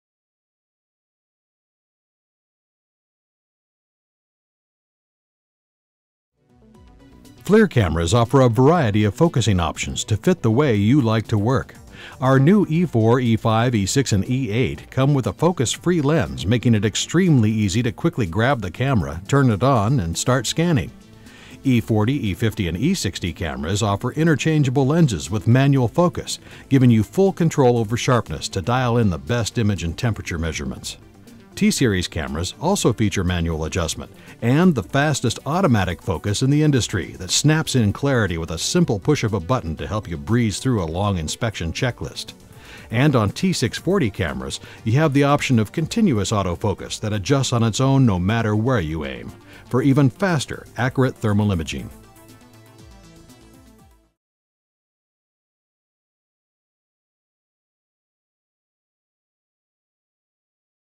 Flir E4 Infrared Camera Interchangeable lenses
Flir E4 Infrared Camera Thermal Imager MSX Enhancement 80 x 60 9Hz

The Flir E4 features Multi Spectral Dynamic Imaging for greater detail in your thermal inspections.

The focus free lens & easy button navigation method to on screen saved JPEGS, imaging modes, settings and measurement tools make The E4 Thermal Imaging Camera incredibly easy to use, even by pressing a single button with a gloved thumb. Also featured is an on screen center spot meter to measure temperatures. You can also view MSX, visual, thermal images & simultaneously store them for later analysis with FLIR Tools software.